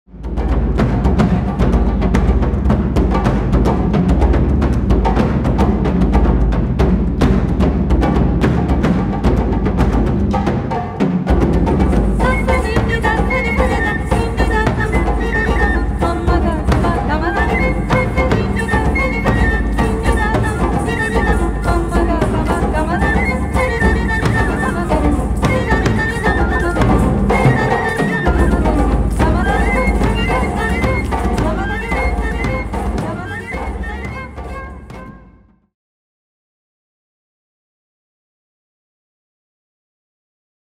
Kalaa Utsavam 2024 | Chi Udaka - Trailer 2 (23 & 24 Nov 2024)
Experience the deep earthiness of taiko drumming, symbolising earth (“chi”), and the graceful flow of Indian classical dance, representing water (“udaka”), in this energetic live performance. Delicately balanced between drama and elegance, this performance unites the resonant taiko drums of Taikoz, the soulful shakuhachi flute, the rich tones of the cello and classical Indian vocals with Lingalayam’s contrasting bharathanatyam and kuchipudi dance styles.